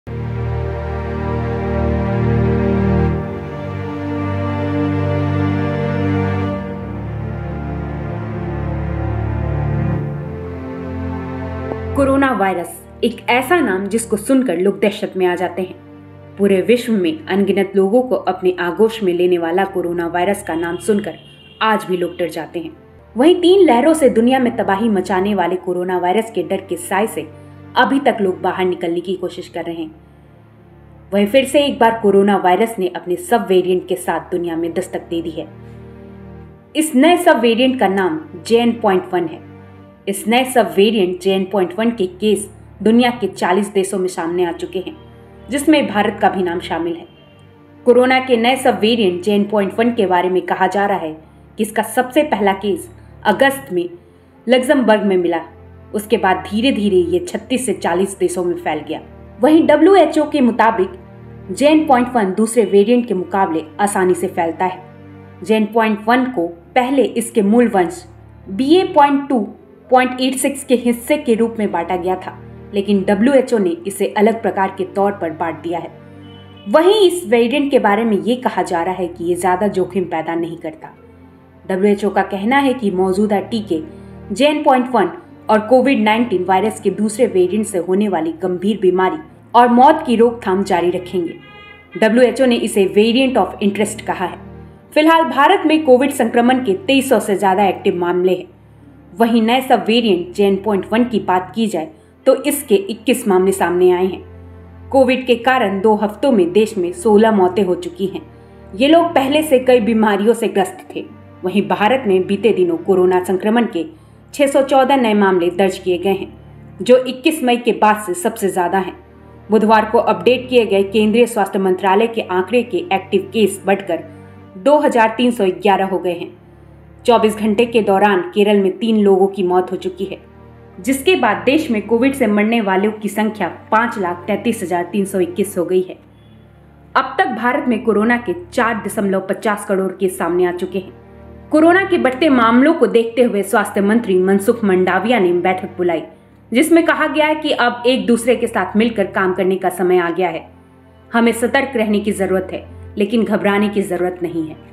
कोरोना वायरस के नए सब वेरिएंट JN.1 ने दी दस्तक
कोरोना वायरस एक ऐसा नाम जिसको सुनकर लोग दहशत में आ जाते है. पूरे विश्व अनगिनत लोगों को अपने आगोस में लेने वाला कोरोना वायरस का नाम सुन कर ही लोग डर जाते है.